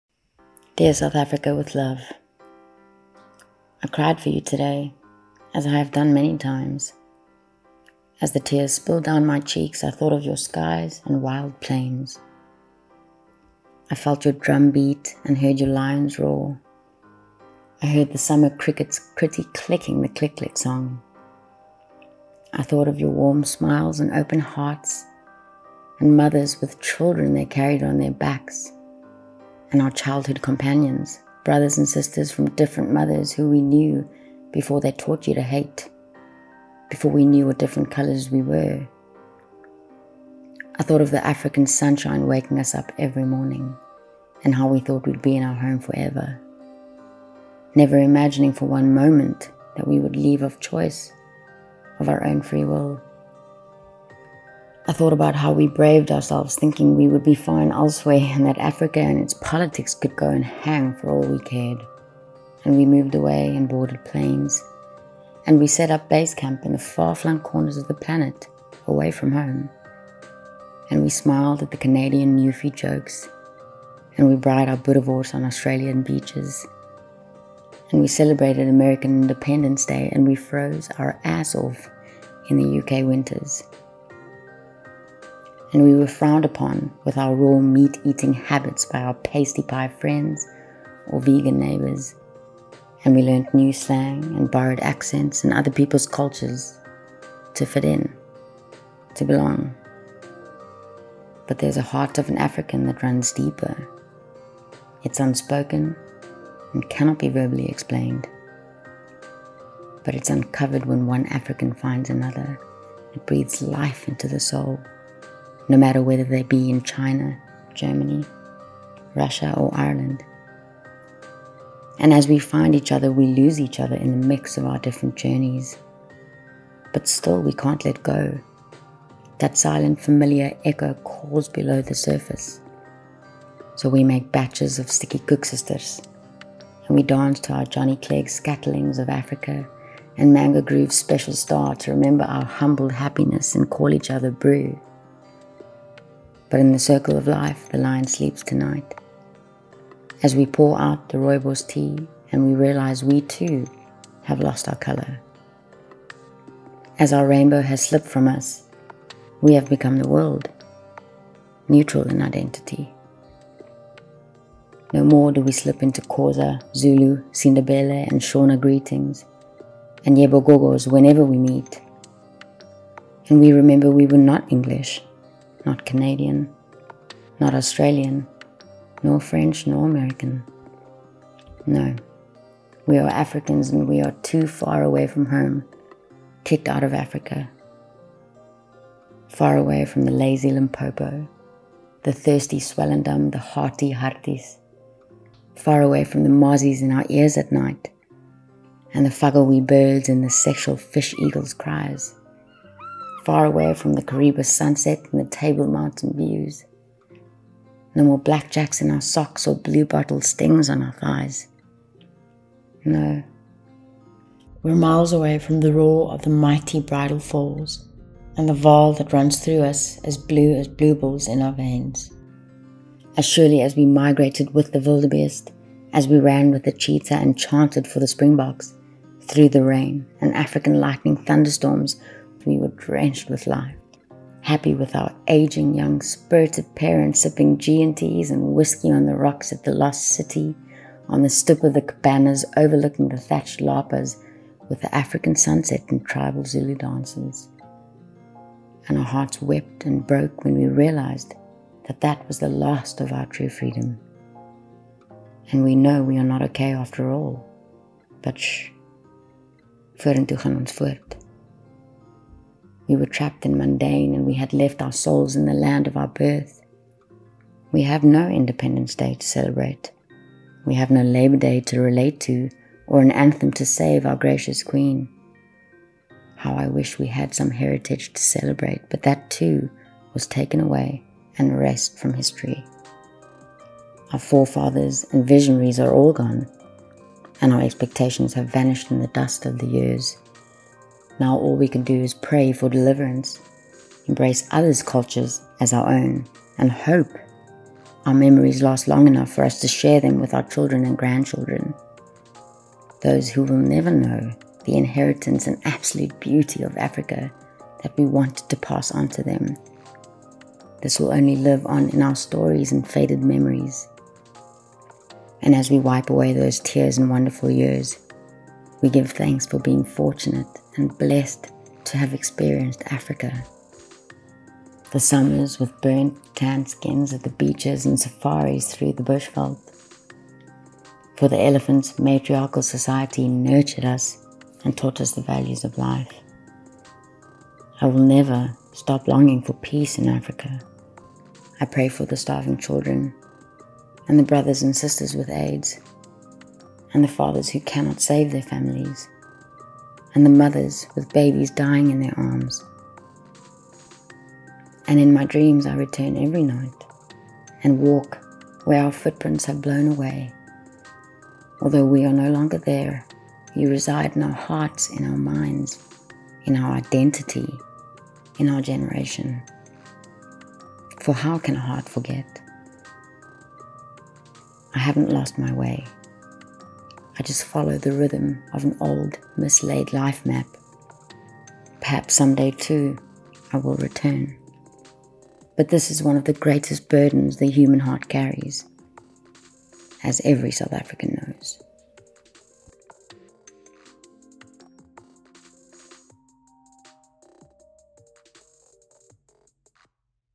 An African ‘kicked out of Africa’: a departed Saffa’s heartbreak
The valiant vulnerability demonstrated in this anonymous South African poem will undoubtedly speak to the affliction carried by countless South Africans. So often, those South Africans who have made the decision to depart our dear country will go on to vehemently report their decision to do so was unquestionably correct. Their message – usually extended with a laundry list of everything that they do not miss, everything that they no longer need to concern themselves with – is, while realistic, almost too overt. Notwithstanding the very real advantages they enjoy elsewhere, it is hard not to wonder whether they, perhaps, are so persistent in their advocacy of emigration because any deviation therefrom would be too painful. While the author of this poem is anonymous, the heartbreak felt by him or her over leaving South Africa is clearly identifiable.